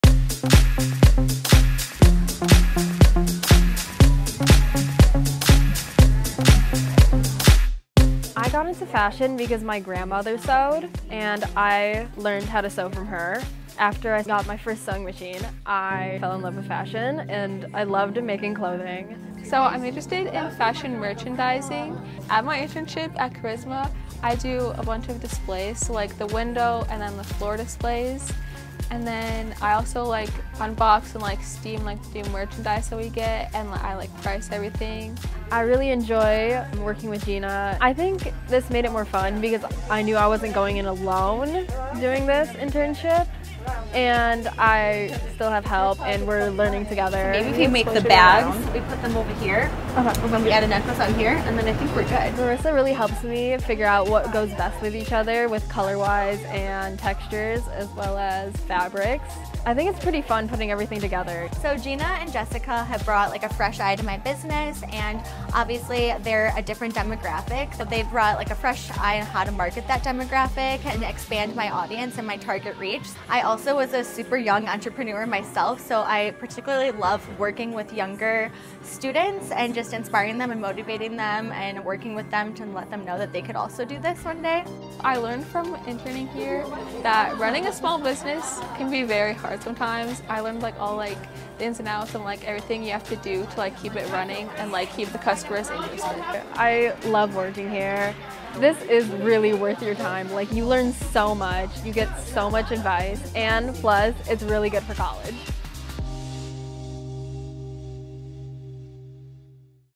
These Summer Interns Have a Passion for Fashion
Our students are discovering their futures through D214 Summer Internships. Buffalo Grove High School juniors Gina and Jessica have a passion for fashion and are learning how to run a business at Charisma Clothing Boutique in Arlington Heights!

Internships are available year-round to all D214 students.